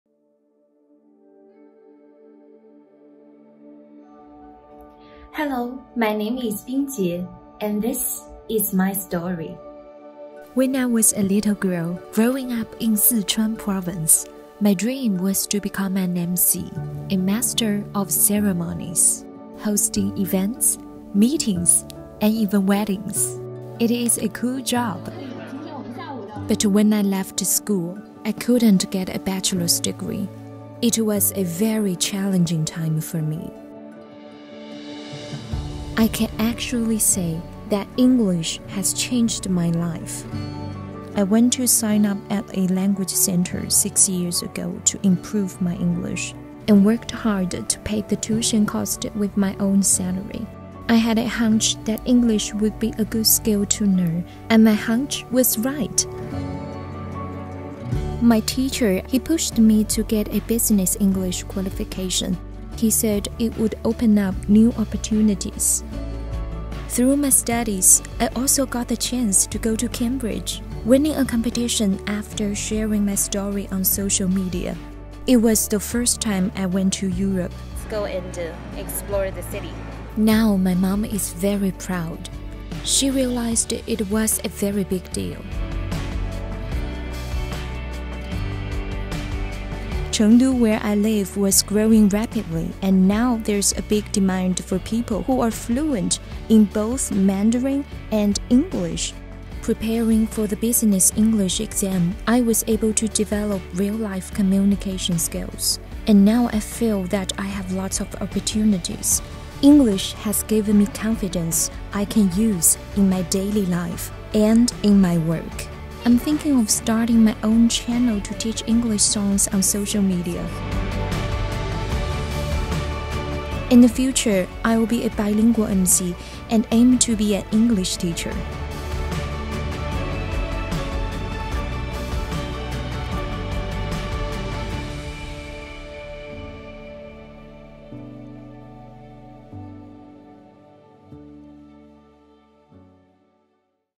See how English has changed BingJie's life
English improves people's lives, one story at a time. BingJie is now building a career as a bilingual Master of Ceremonies and aiming to become an English teacher. Watch a short film about BingJie and see how English has changed her life.